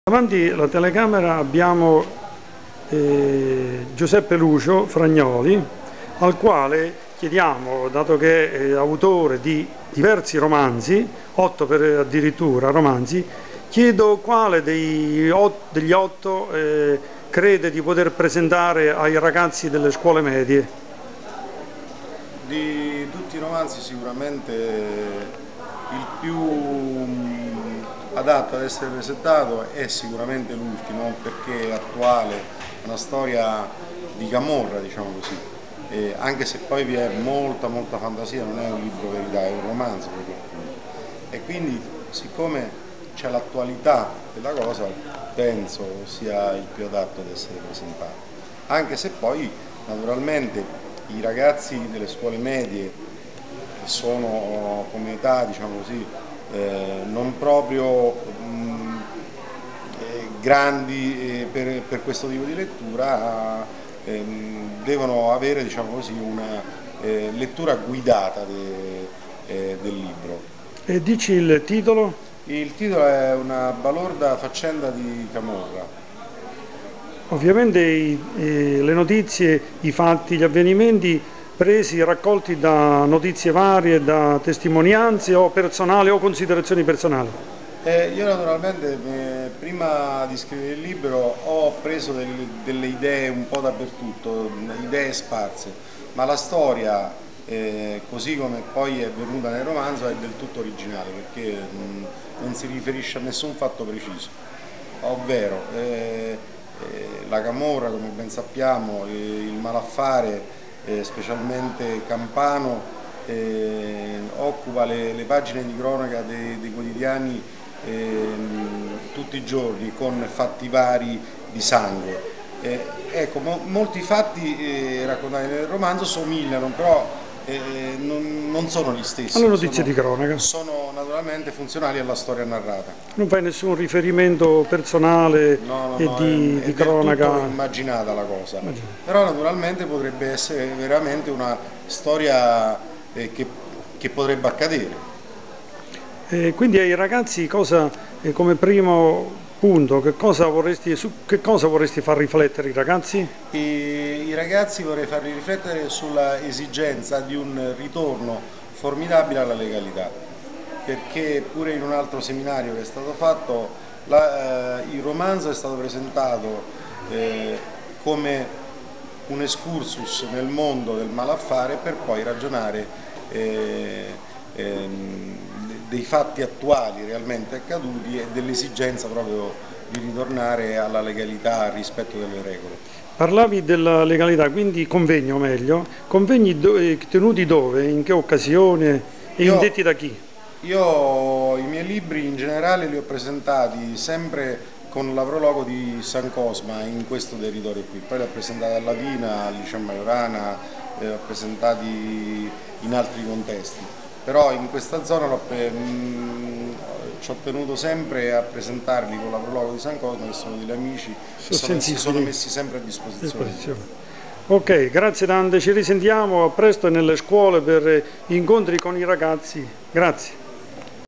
CAMORRA, intervista all'Autore del Libro
Intervista a Fragnoli, autore di 'Una balorda faccenda di Camorra' al Papardò di Gaeta (agosto 2008)(video intervista di GaltFede)